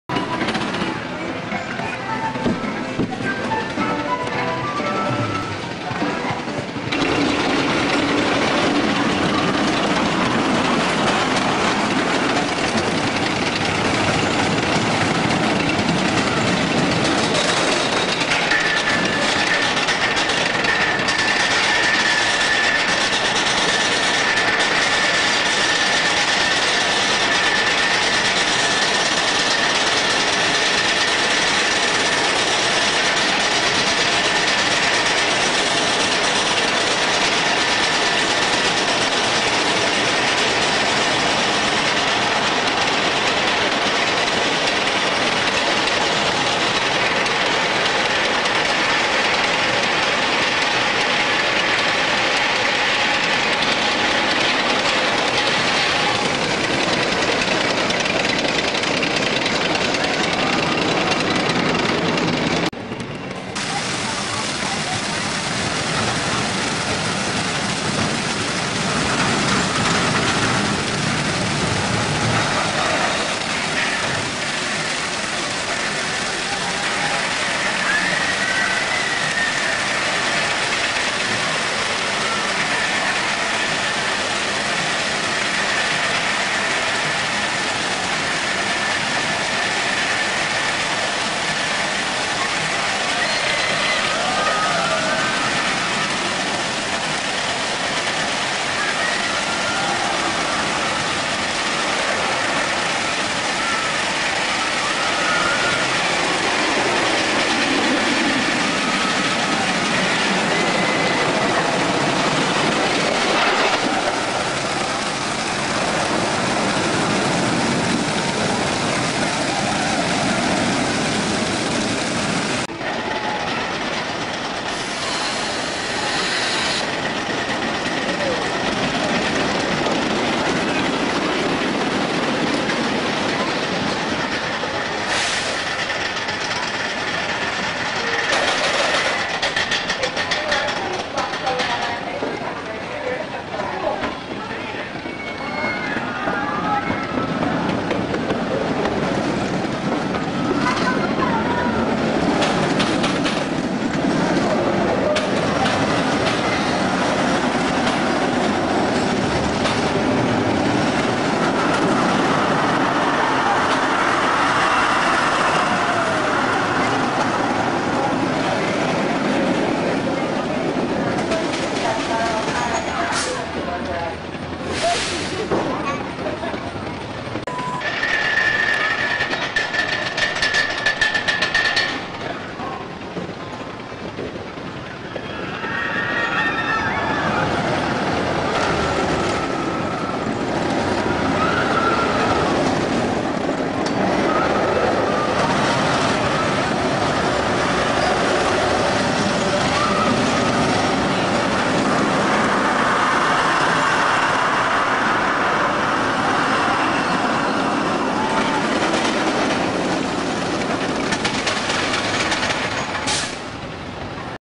dragon adventureland iowa
I took these videos when i was there in august 2001